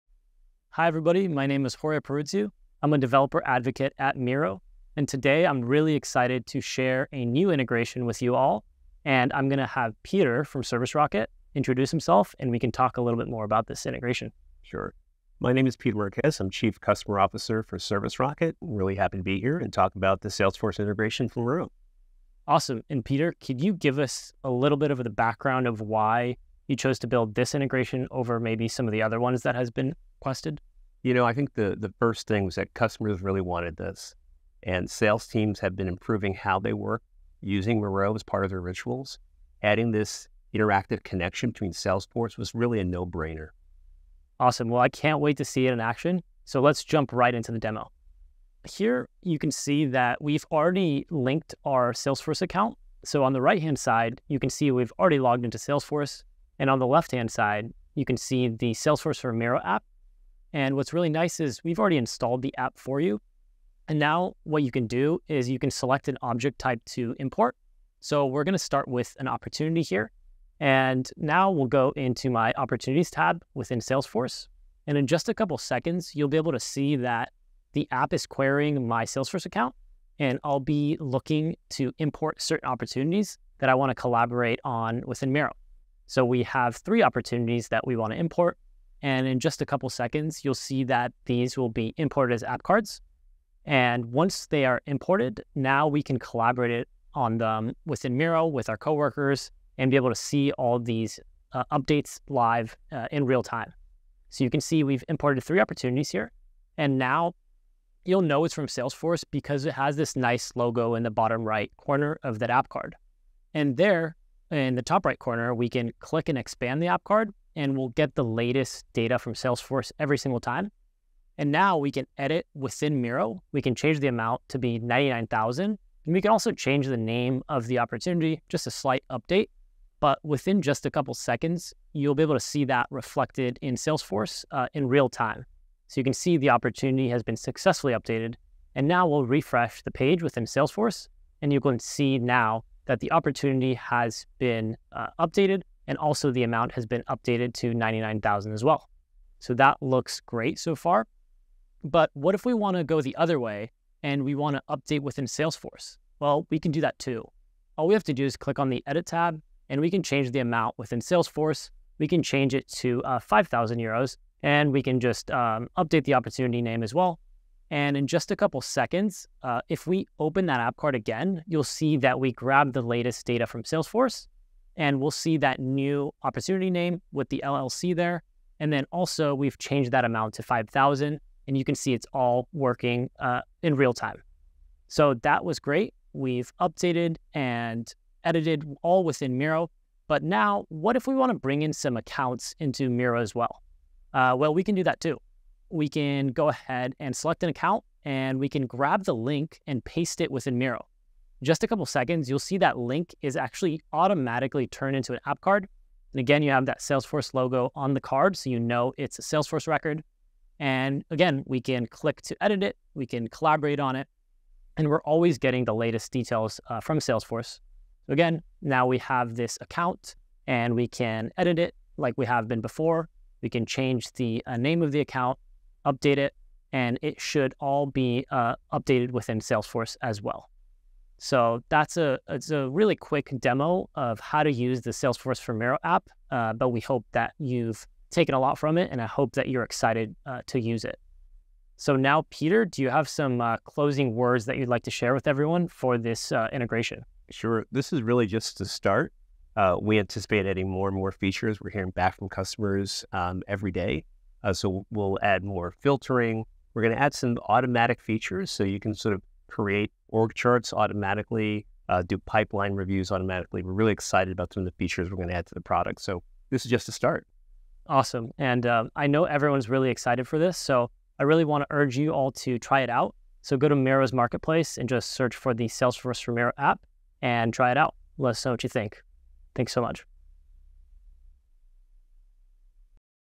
Salesforce Integration for Miro: 2 Way Data Sync
The Salesforce integration for Miro integrates a real time, 2 way data sync between Salesforce and Miro. This video will show you how to import Salesforce opportunities, accounts, and contacts directly from your Salesforce instance. Once you’ve imported your Salesforce Objects within Miro, you can then edit, update, and collaborate on Salesforce Objects directly in Miro without leaving your Miro board. All updates you make in Miro will be updated in Salesforce in real time. At the end of the video, you will learn a bit more about enhancements which will be made to the app in the future, such as more filtering capabilities.

Learn from Horea Porutiu, Senior Developer Advocate at Miro and Peter Marquez, Chief Customer Officer at ServiceRocket, about the full functionality of the Salesforce for Miro app and the value it brings to Sales teams working in Miro.

ServiceRocket is a Miro Marketplace Partner and a technology solutions partner trusted by thousands of organizations.

00:00 - Intro
00:22 - Why Salesforce Integration for Miro
00:48 - Salesforce for Miro app intro
01:09 - Salesforce for Miro app Import Opportunities
02:00 - Edit Salesforce Opportunities in Miro
03:10 - Import Accounts from Salesforce URL into Miro
03:30 - Demo wrap up
04:01 - Closing remarks for integration